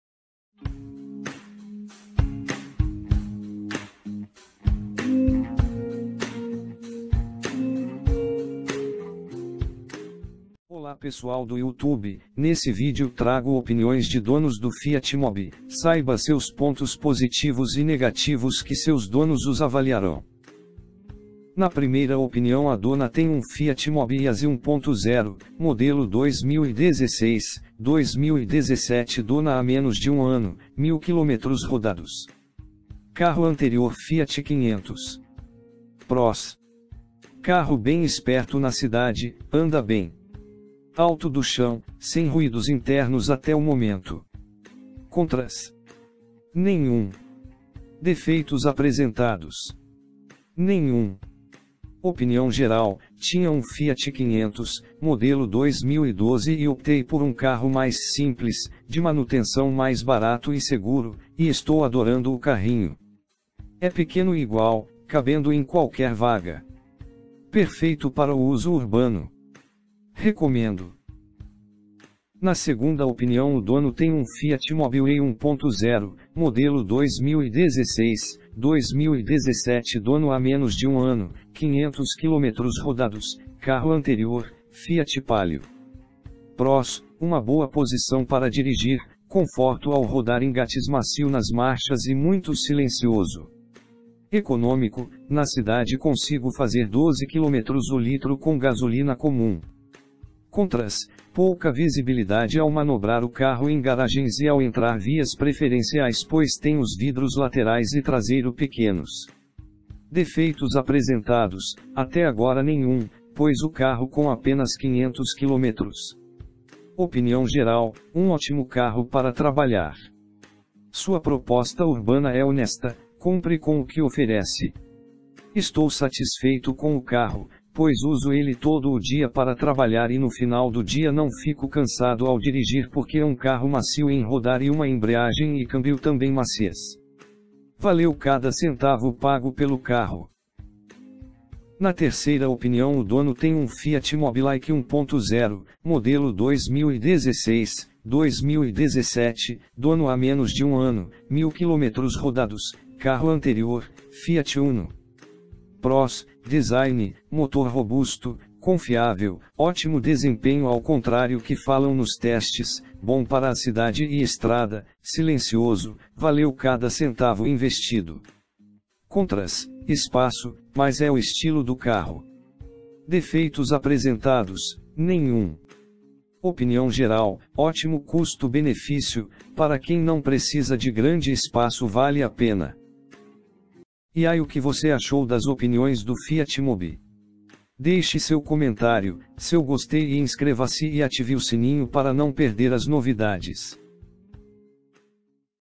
Fiat Mobi 2017 - Opnião do Dono (Top Sounds) | Top Carros
Olá pessoal do YouTube, nesse vídeo trago Opiniões de donos do Fiat Mobi!!

Created by VideoShow Top Carros